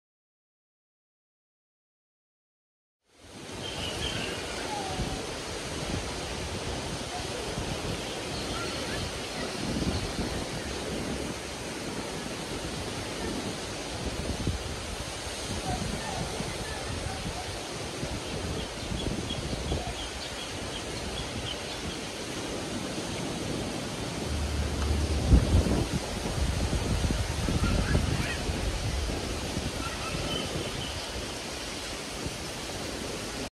เสียงทะเลหัวหิน เขาเต่า นั่งชิวๆ - รีวิวไปเรื่อย
นั่งชิวๆข้างทะเลหัวหิน ต้นปี2020
หาดตรงเขาเต่า